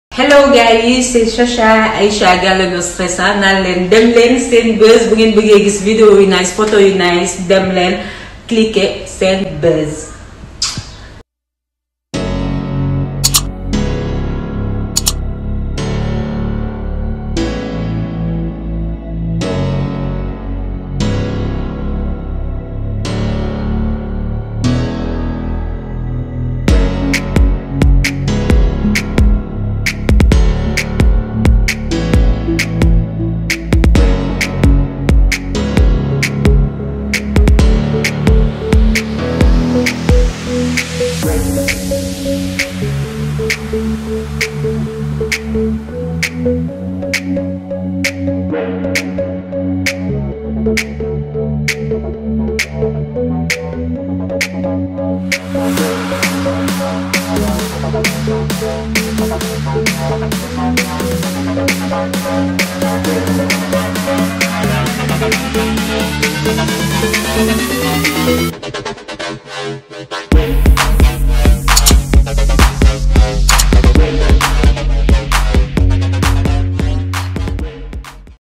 (PHOTOS) Sophia de la 2stv une étoile montante de l'animation de la télévision sénégalaise
Pour plus de visibilité de vos shooting appelez au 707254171 on le publie gratuitement.
EN HAUTE DÉFINITION :